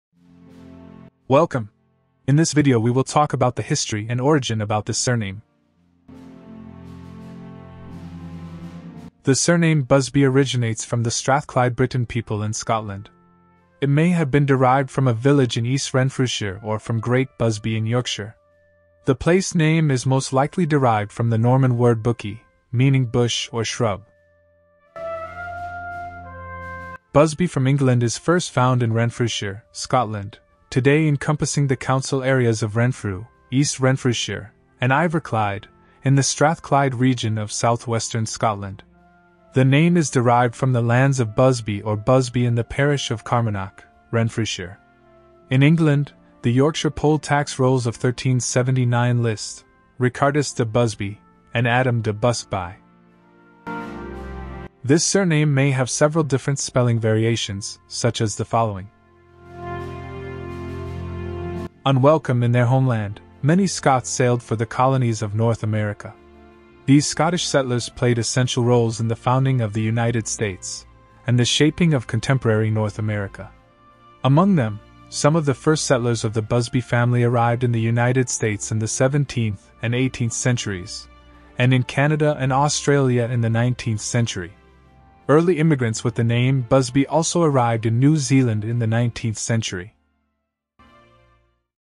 Busby from England 🏴󠁧󠁢󠁥󠁮󠁧󠁿 - Meaning, Origin, History & Migration Routes of Surname
The surname Busby originates from the Strathclyde-Briton people in Scotland. It may have been derived from a village in East Renfrewshire or from Great Busby in Yorkshire. The place name is most likely derived from the Norman word 'buki', meaning 'bush' or 'shrub.'

Busby from England is first found in Renfrewshire, Scotland, today encompassing the Council Areas of Renfrew, East Renfrewshire, and Iverclyde, in the Strathclyde region of southwestern Scotland. The name is derived from the lands of Busby or Busbie in the parish of Carmunnock, Renfrewshire. In England, the Yorkshire Poll Tax Rolls of 1379 list: Ricardus de Busby, and Adam de Buskeby.